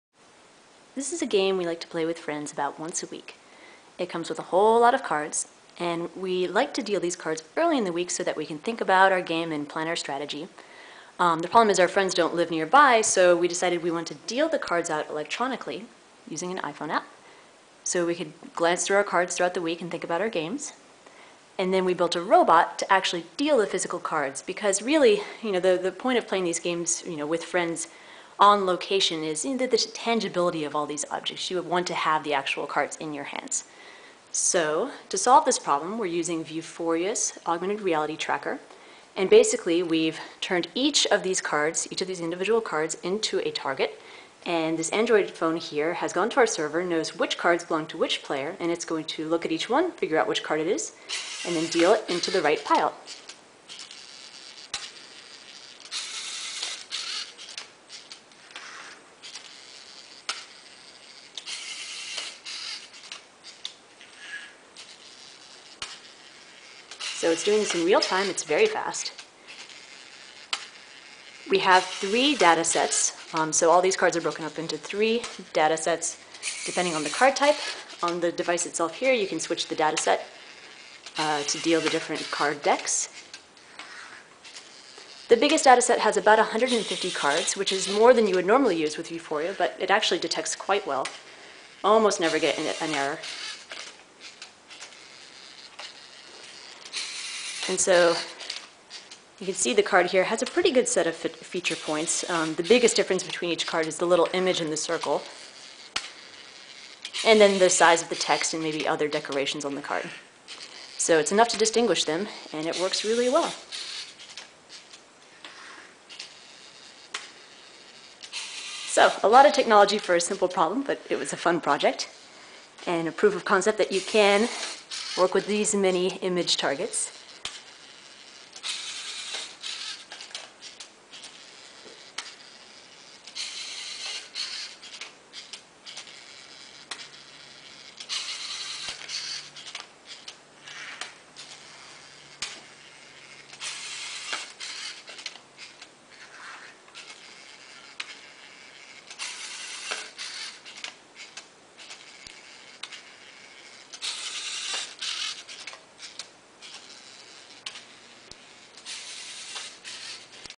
Agricola Dealing Robot
Here's a robot we made using Lego NXT to deal Agricola cards.  We wrote an iPhone app for dealing a new game and letting players browse through their cards.  Then the Android device on the robot connects to our server and downloads the hands.  It uses Qualcomm's Vuforia augmented reality software to distinguish each card in the phone's camera stream, then deals it to the correct pile.  This was a pet project for fun, we realize this isn't the most efficient way to solve this problem but it was a great excuse to play with all this tech!